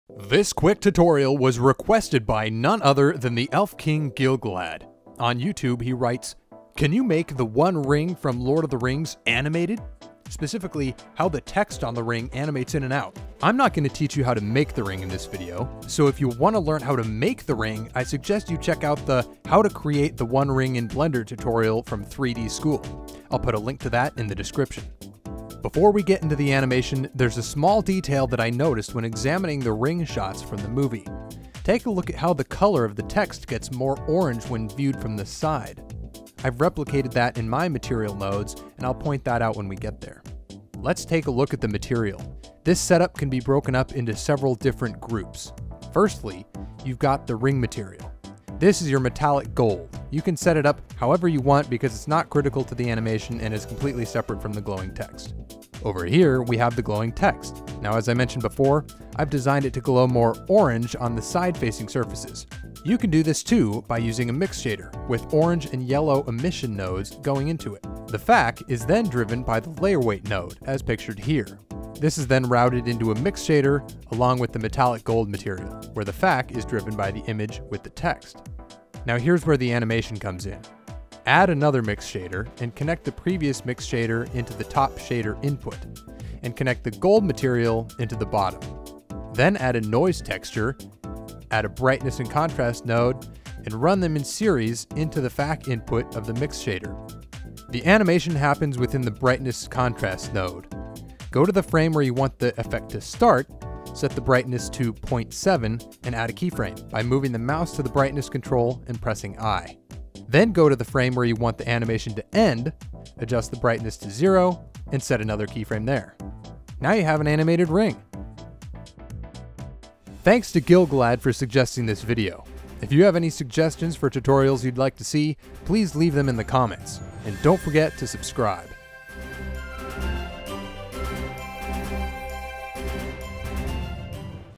Blender Tutorial: Animated Text on The One Ring
3dschool "how to create ''the one ring'' in blender" tutorial

My assets came from:

HDRI